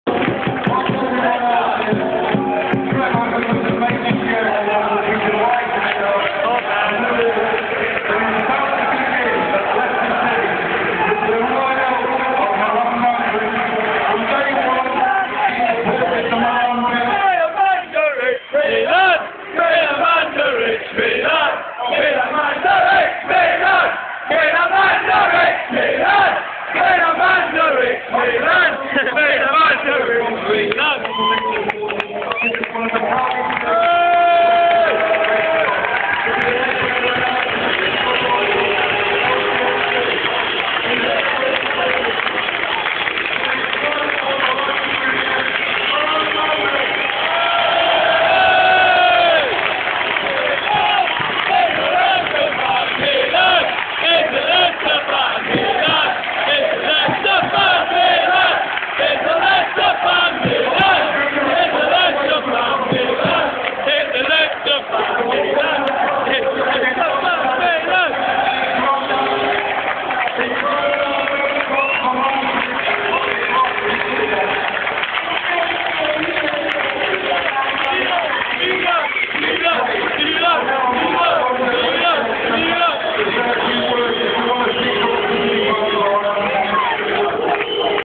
leicester welcomes milan mandarich
milan mandarich walks out befor the game